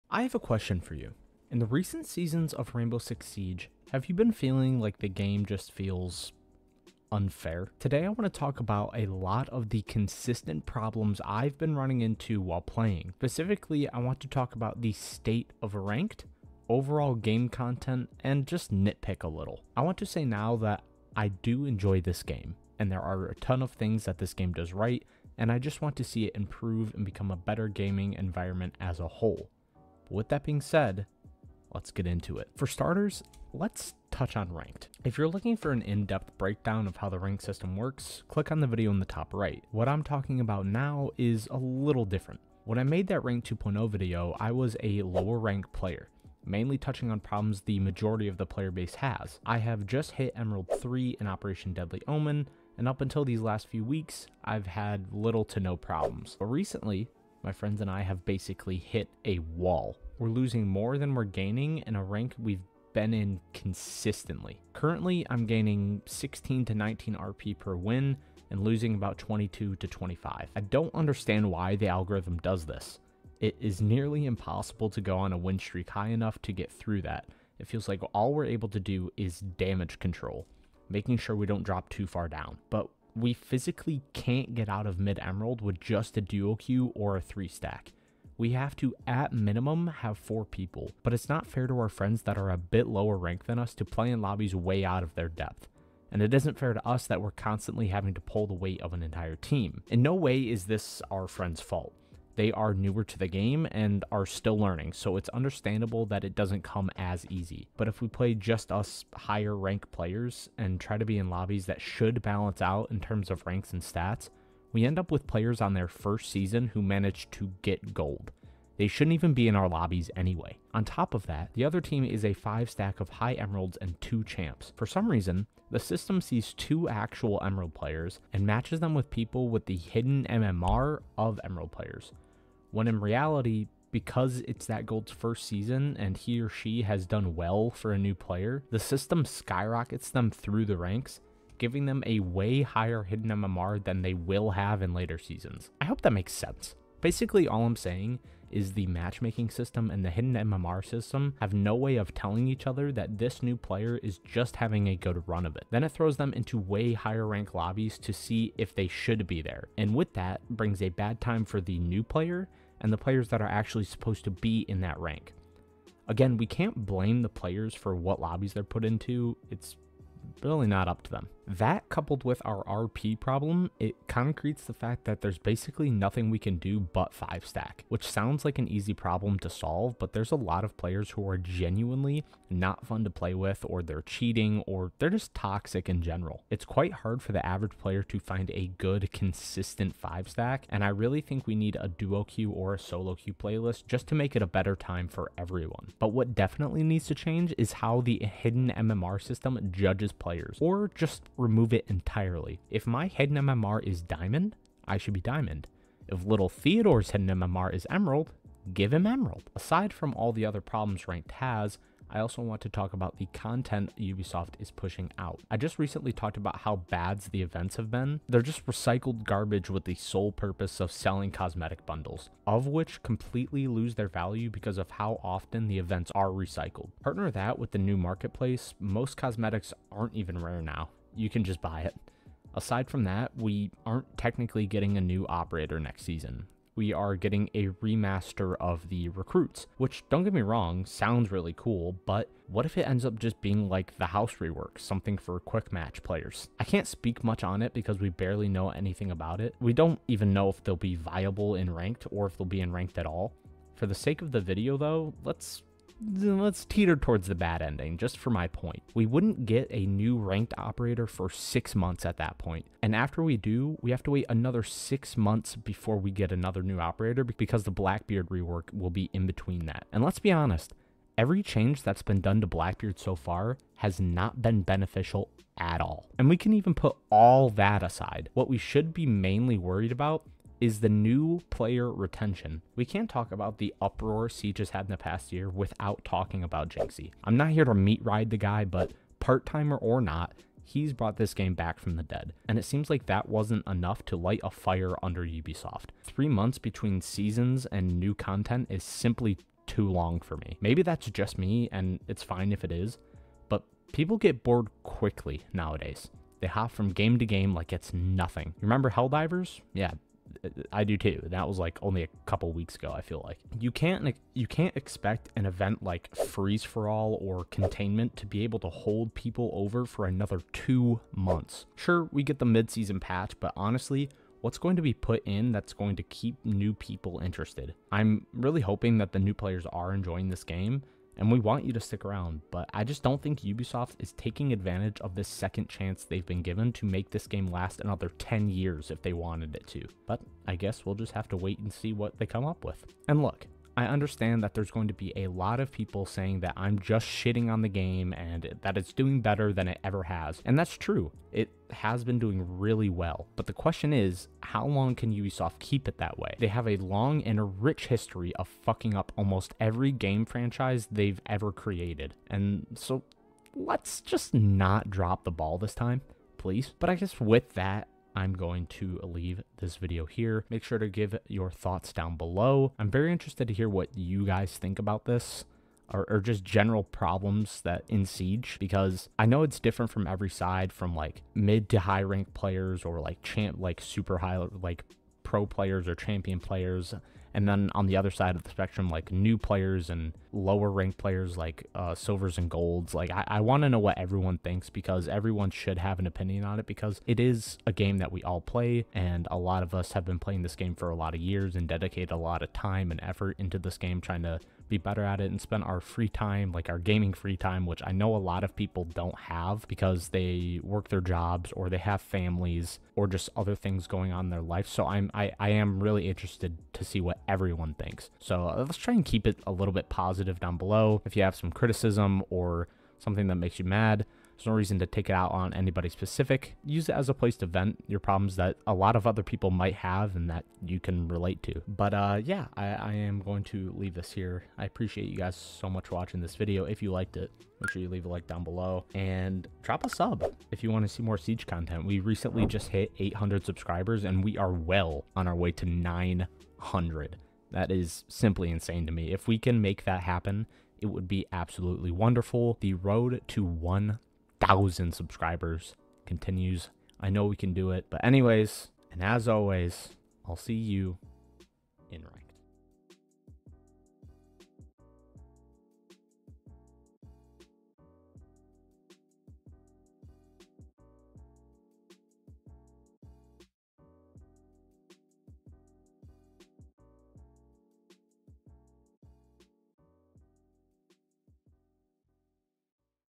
How Long Will Siege Last?
Today we are talking about the current state of Rainbow Six Siege. We touch on topics like the Ranked system, the content Ubisoft is pushing out, and the overall experience for new players.